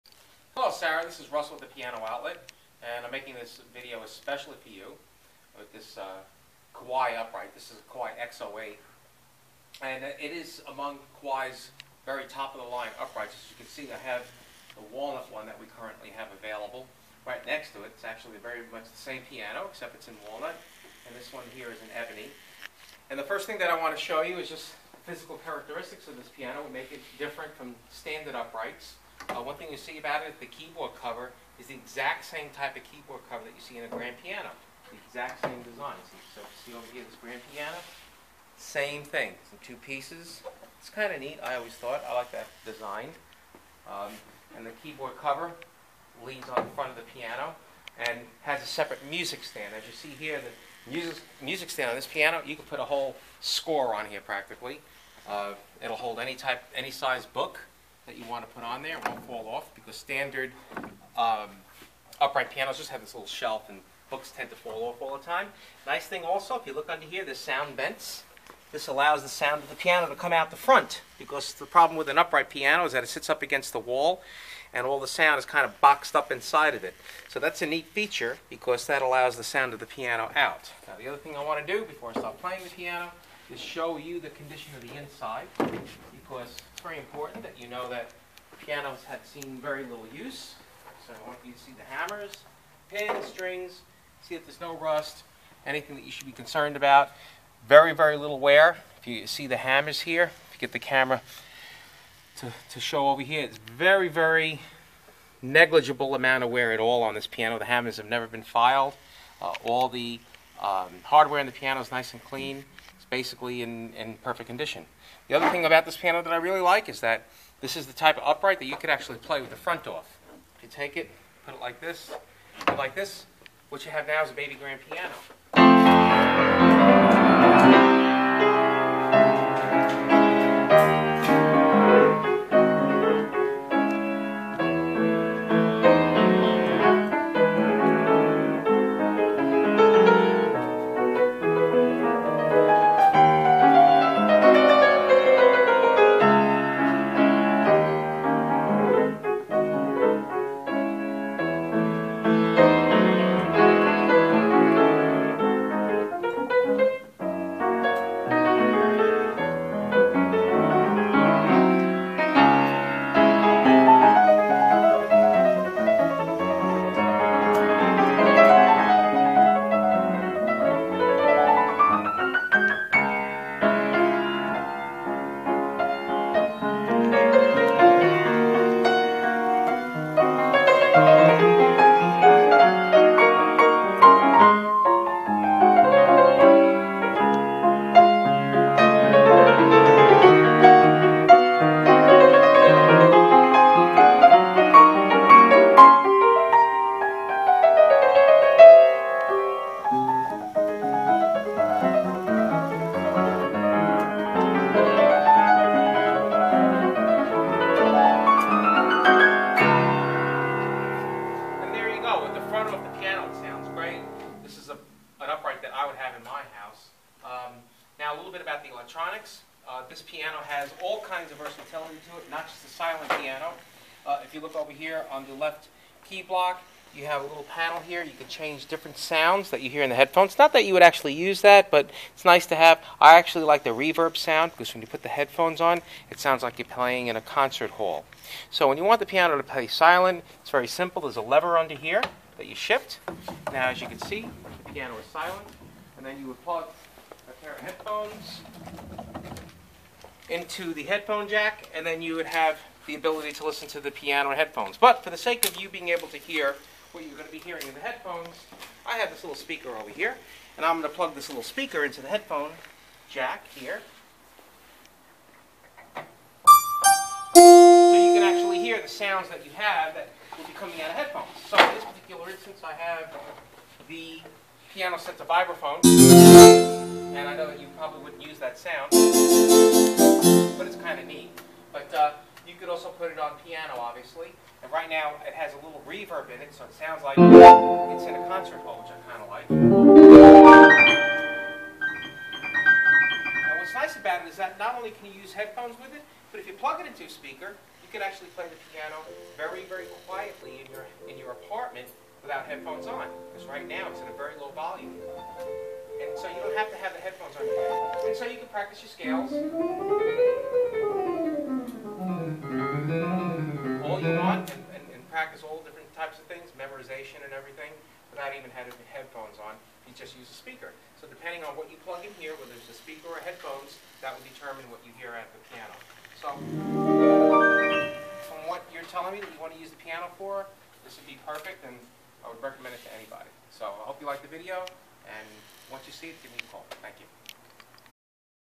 Kawai Silent Studio Digital Upright for Sara
Russell demonstrating quickly a georgeous studio upright piano made by Kawai. If you would like to contact PianoOutlet, or obtain more information about our inventory & services, visit our